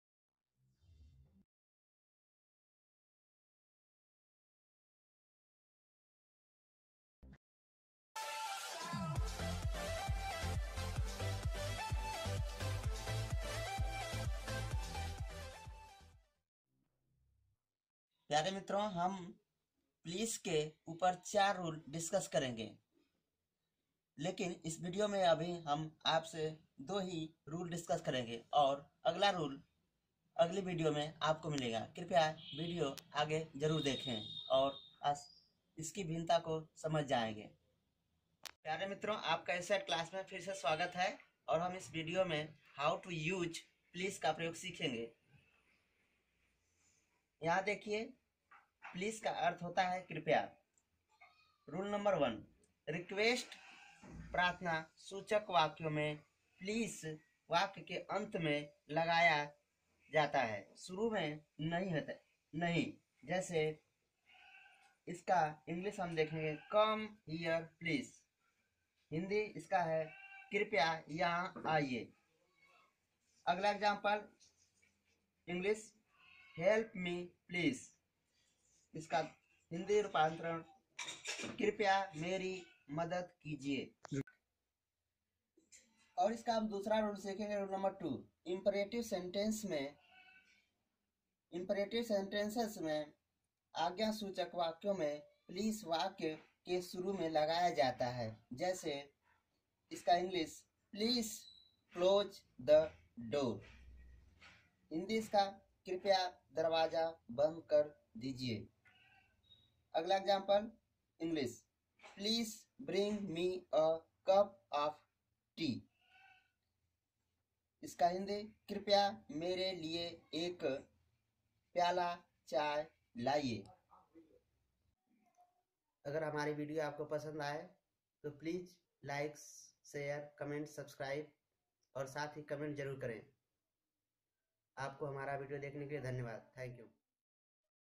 How to use "Please" Please का एकदम अद्भुत प्रयोग देखे।
Please का बेहतरीन प्रयोग और चार नियम जुड़ा हुआ।